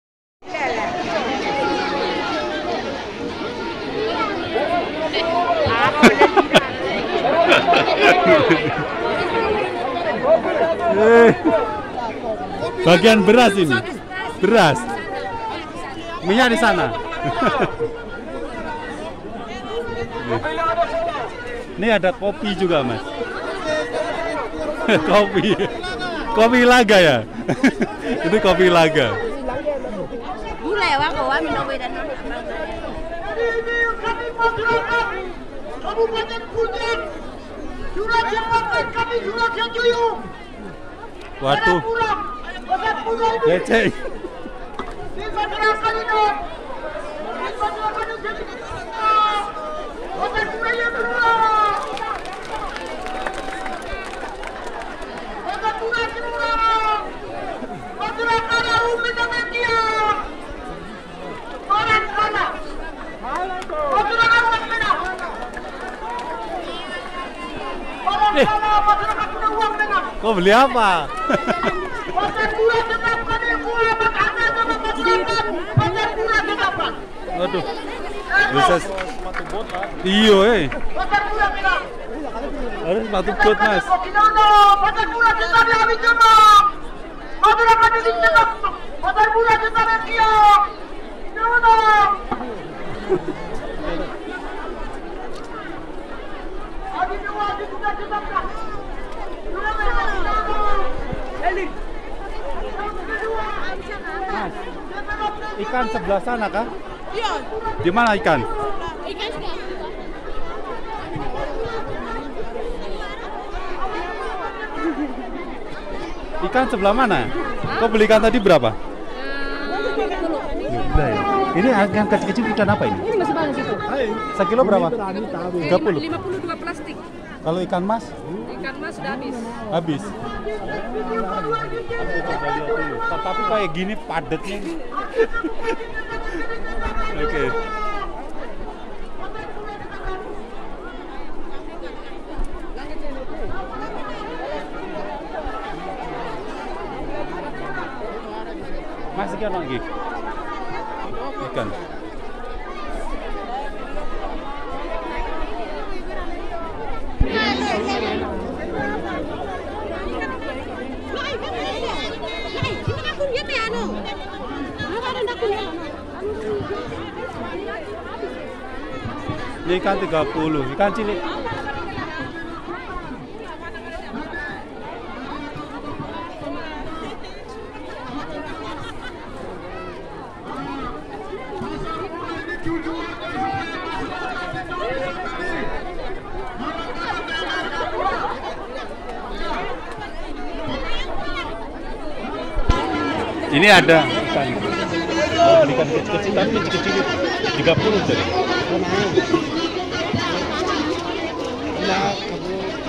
Pasar murah di lapangan trikora Ilaga puncak papua
pasar murah di ilaga diselengarakan di lapangan